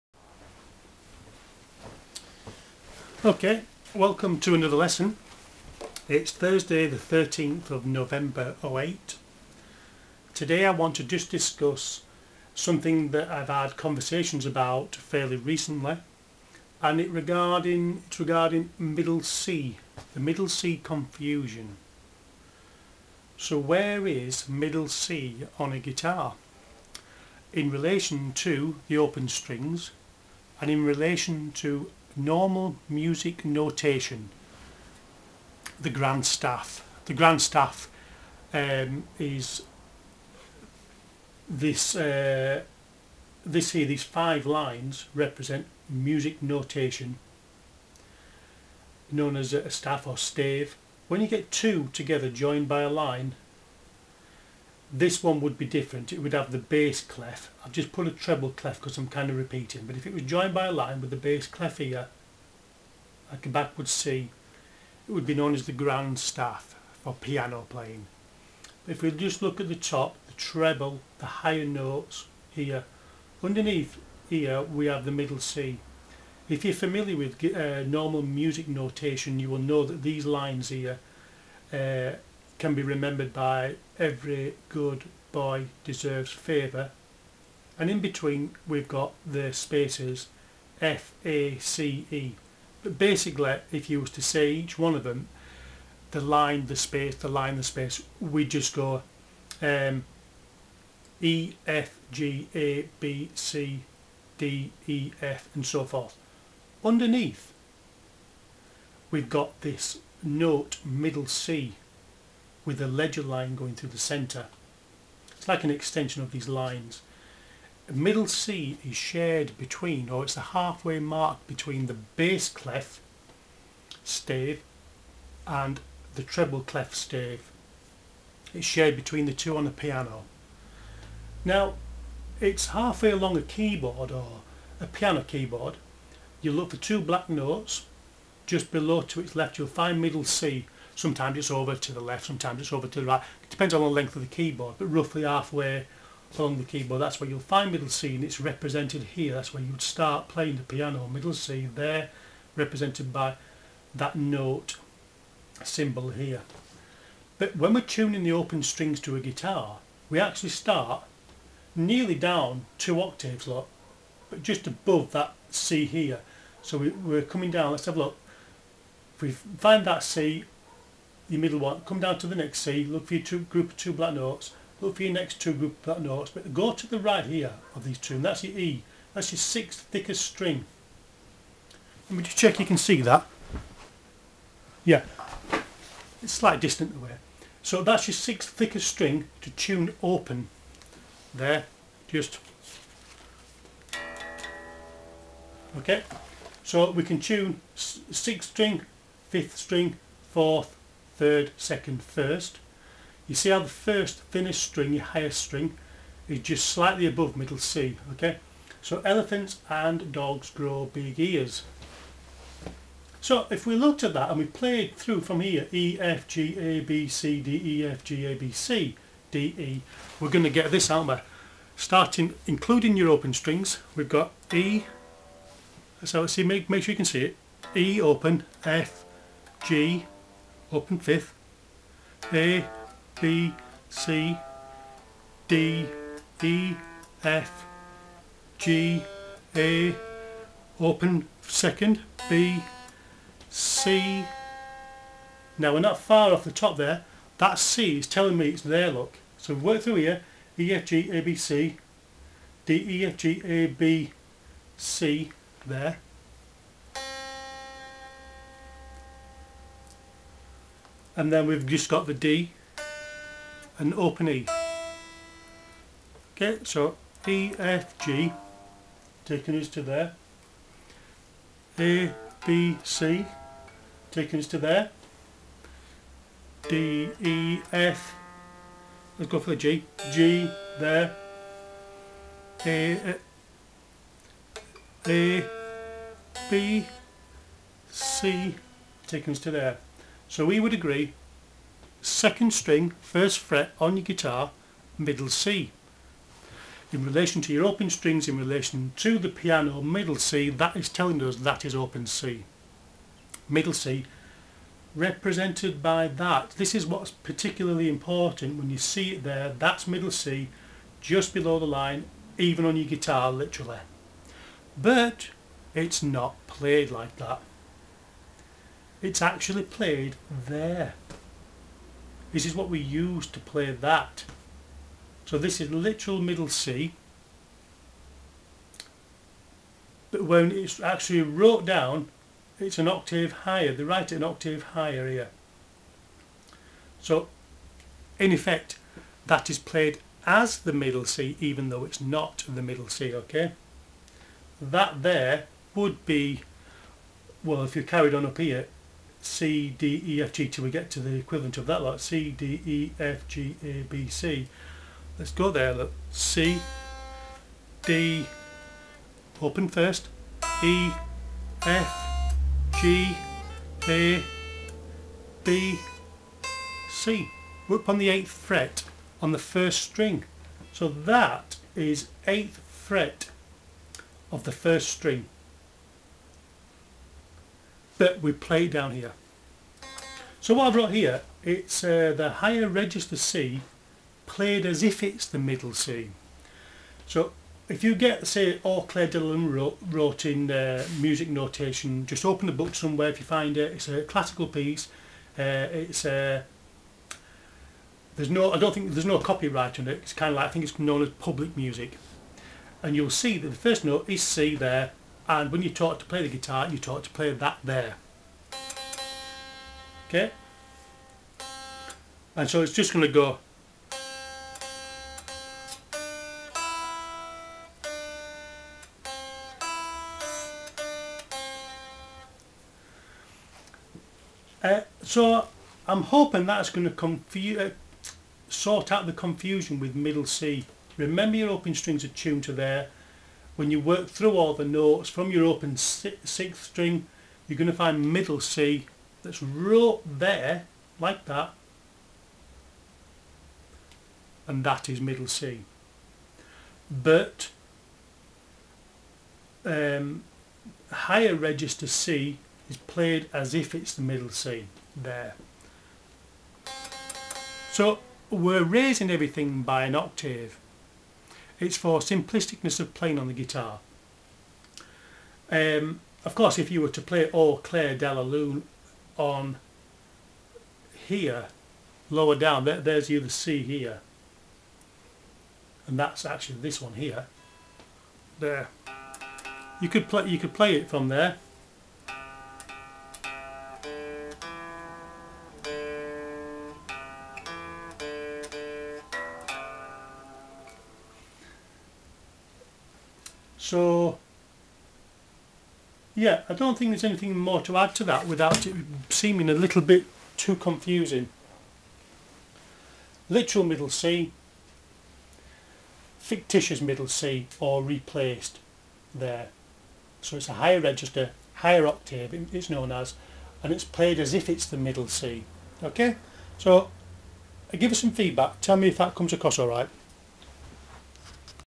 A Lesson To Try Explain The Confusion Over Middle C In Music Theory.
A Lesson To Try Explain The Confusion About Middle C